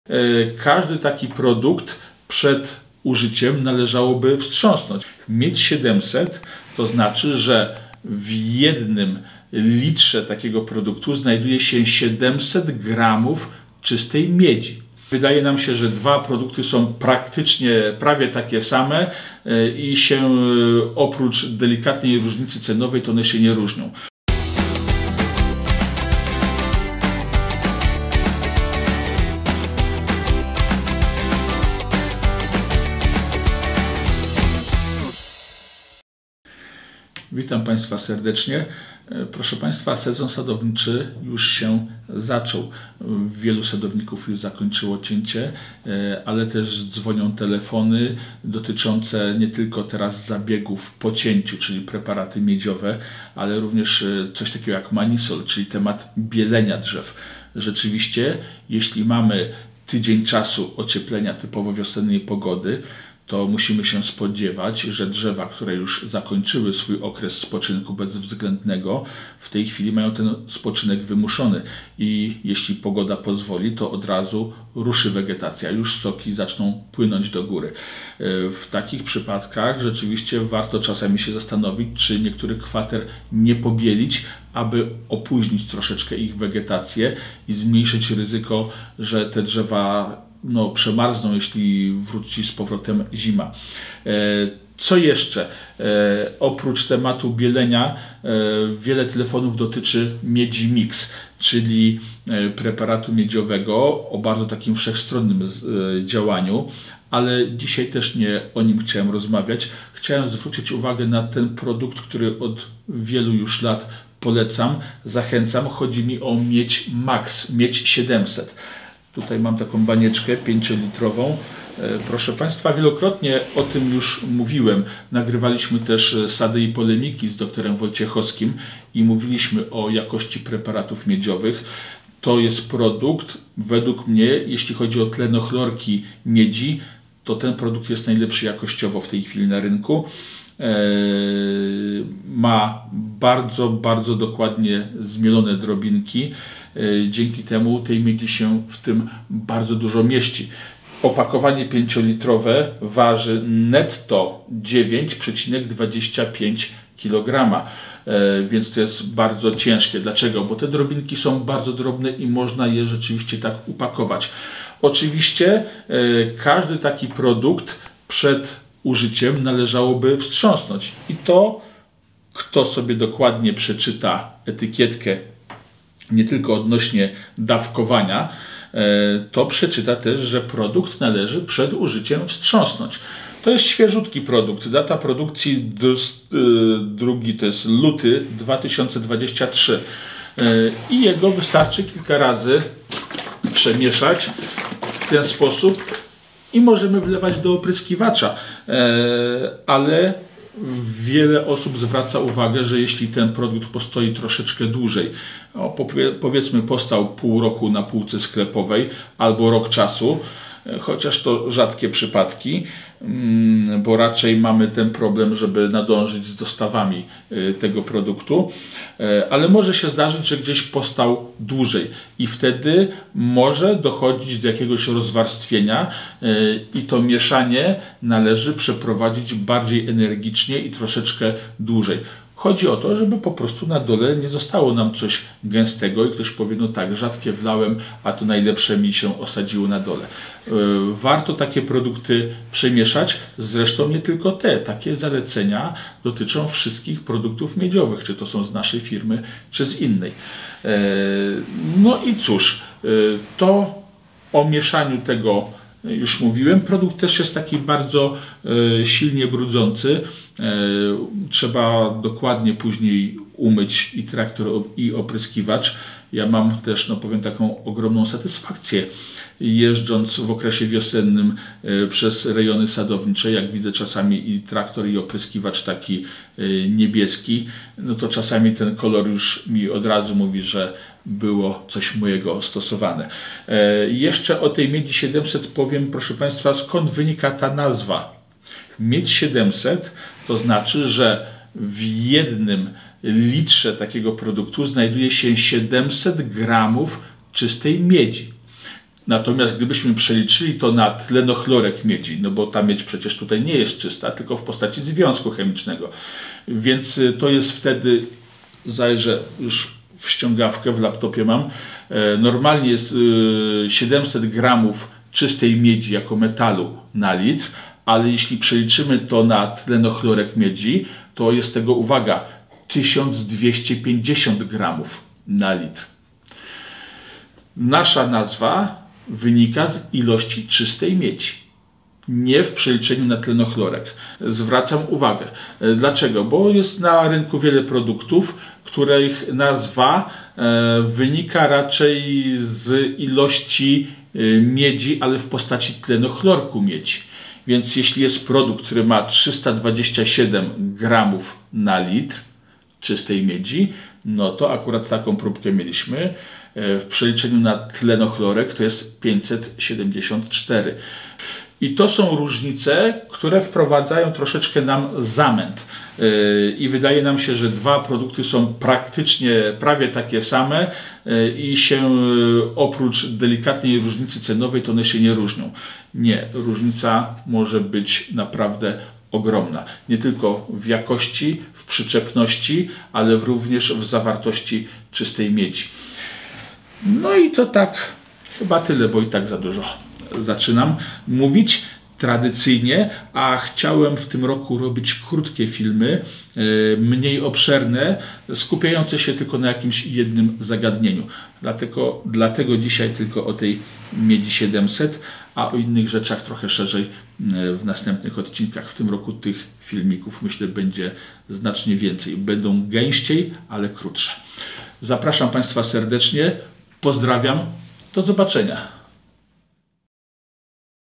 MIEDŹ MAX 700 - Nie zapomnij wstrząsnąć przed użyciem
Sezon sadowniczy rozpoczął się na dobre. Jednym z tematów, który zajmuje sadowników jest kwestia bielenia drzew w celu opóźnienia wegetacji i uniknięcia uszkodzeń w wypadku przymrozków. Rozwiązaniem jest tu produkt Revival.

Drugi ważny temat to zabiegi miedziowe po cięciu. Wybierając produkt miedziowy warto  zwrócić uwagę na całkowitą zawartość miedzi. W produkcie MIEDŹ MAX 700 jest aż 700 g czystej miedzi na litr, co odpowiada 1250 g tlenochlorku miedzi na litr.

Ze względu na dużą gęstość MIEDZI MAX 700, nawóz ten należy wstrząsnąć przed użyciem. Jest to szczególnie istotne, jeśli produkt stał nieco dłużej w sklepie czy pośród gospodarskich zapasów.

REVIVAL - nawóz zawierający wapń i aminokwasy, który tworzy na roślinach białą powłokę odbijającą promienie słoneczne

MIEDŹ MAX 700 - nawóz miedziowy o doskonałej przyczepności, malutkich cząsteczkach i bardzo dużej zawartości miedzi (700 g/litr)